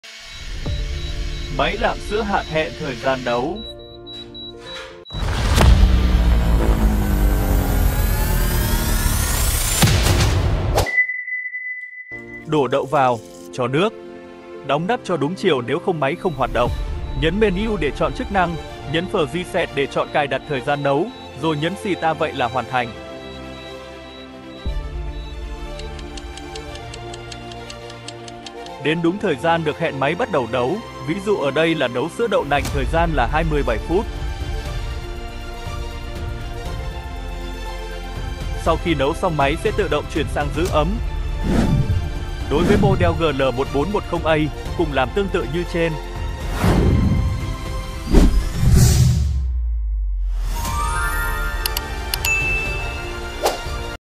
Hướng dẫn sử dụng máy làm sữa hạt Gali
Bạn mua máy làm sữa hạt, bạn chưa biết sử dụng ra sao hoặc cần thêm kiếm thức về sản phẩm này. Mời bạn xem hết video hướng dẫn của chúng tôi, để sử dụng được dễ dàng.